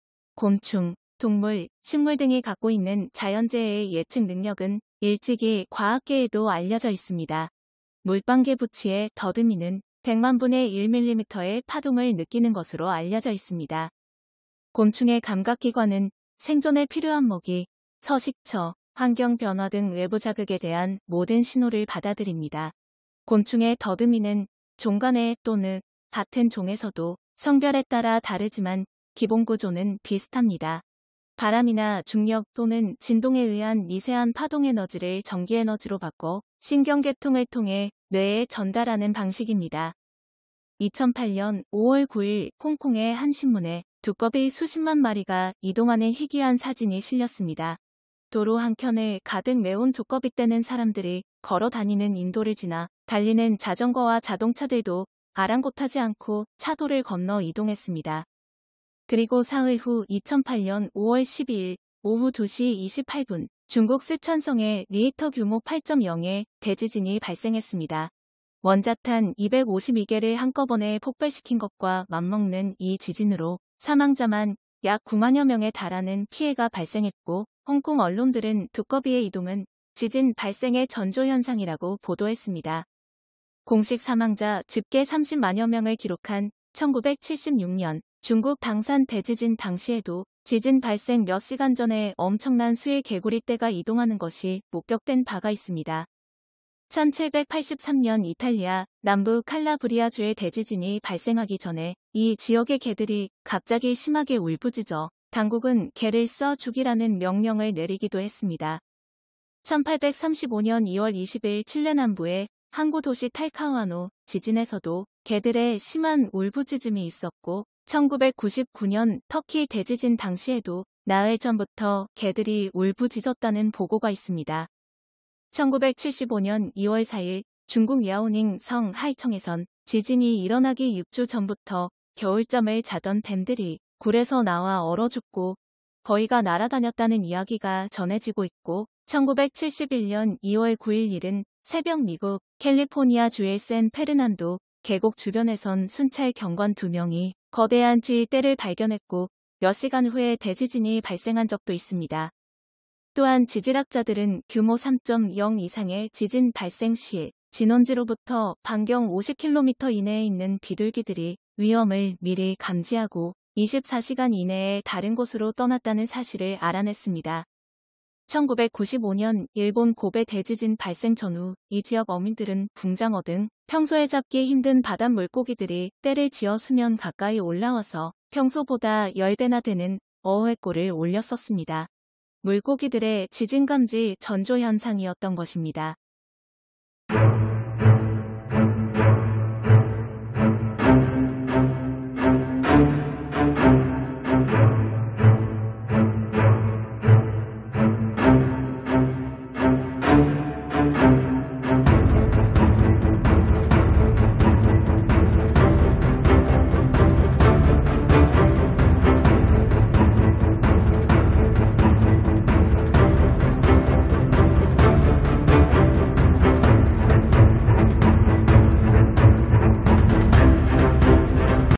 【일본 난리났다! 대지진전조현상 두꺼비떼 출몰, 비둘기이동, 지진물고기등장 이탈리아 개들 울부짖어 곤충,식물,동물 이상현상 대지진 전조현상!】
【일본 난리났다! 대지진전조현상 두꺼비떼 출몰, 비둘기이동, 지진물고기등장 이탈리아 개들울부짖어 곤충,식물,동물 이상현상 대지진 전조현상!】

최근 고래들의 집단폐사현상인 스트랜딩(Stranding)현상이 일본에서 자주 목격되고 있으며 이러한 돌고래들의 폐사이후에 강진들이 연이어 발생을 하고 있습니다.
또한, 11월14일 후지산 이상한 구름층, 12일 시즈오카 스프라이트현상, 11월6일 미야기현 강에서 물고기들 떼죽음, 요코하마 비늘구름출현, 2주전에 까마귀떼 울부짖는등의 이상현상들이 발생하고 있습니다.